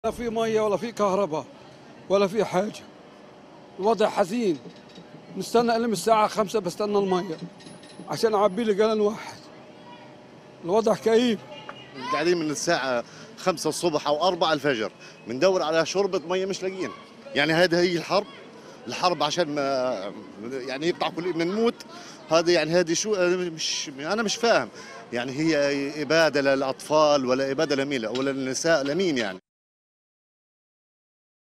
شهادات سكان غزة: إسرائيل تريد قتلنا بالقصف والعطش
لمعرفة أحدث المستجدات.. عبر البث المباشر من هنا ⤵
وبإمكانكم متابعة العربية على منصات التواصل